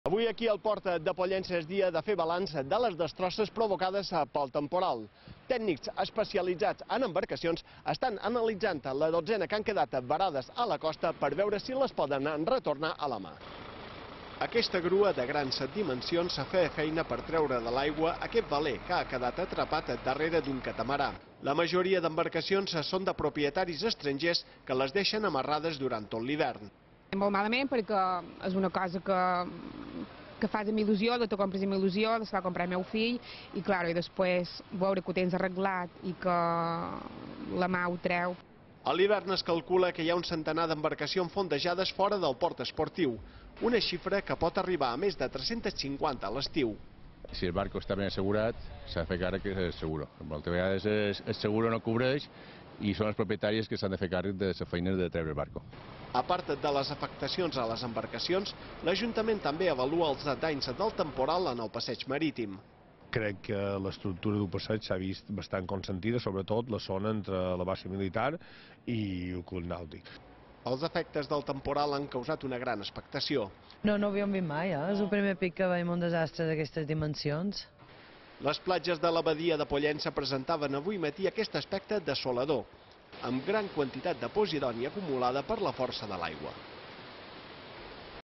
Comencen a retirar la dotzena d’embarcacions varades al Port de Pollença
Tècnics especialistes han fet feina avui per retirar la dotzena d’embarcacions que varen restar varades ahir al Port de Pollença. Bussejadors repassaven els cascs dels vaixells per avaluar els danys que han sofert. Algunes embarcacions les podran treure amb una grua i d’altres les hauran de remolcar. En la majoria dels casos, les assegurances es faran càrrec de les despeses de tornar les embarcacions a la mar.

La majoria d’embarcacions són de propietaris estrangers, que les deixen amarrades durant tot l’hivern. Es calcula que, a l’hivern, hi ha un centenar d’embarcacions fondejades fora del port esportiu, una xifra que pot arribar a més de 350 a l’estiu.

A part de les afectacions a les embarcacions, l’Ajuntament també avalua els danys del temporal en el passeig Marítim. Les platges de la badia de Pollença presentaven avui matí aquest una dessolador, han perdut molta arena i amb gran quantitat de posidònia acumulada per la força de l’aigua.